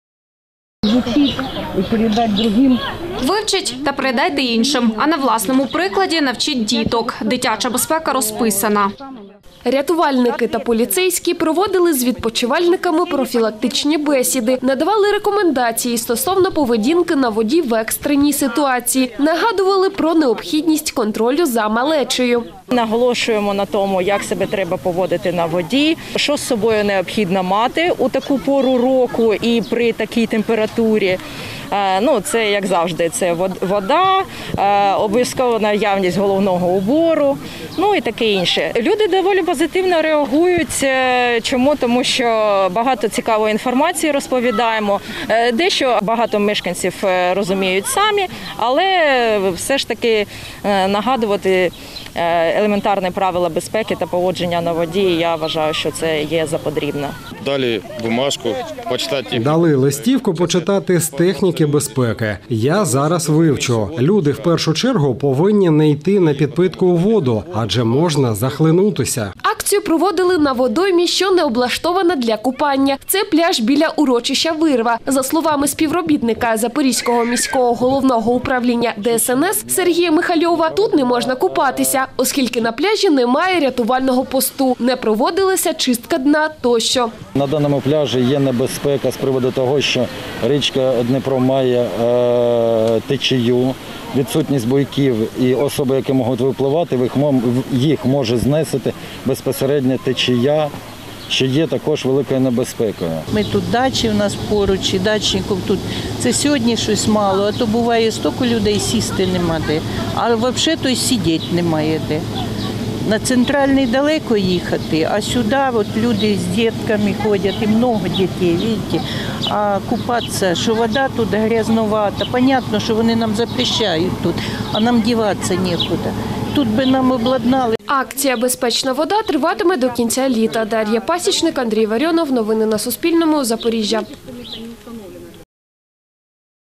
Новини - Акція "Безпечна вода" - 03.07.2020
Запорізькі рятувальники разом з патрульними та представниками Національної поліції провели на одному з пляжів міста акцію "Безпечна вода". Її мета  - попередити людей про правила безпеки поведінки на водних об'єктах. Про перебіг акції - далі у сюжеті журналістів Суспільного.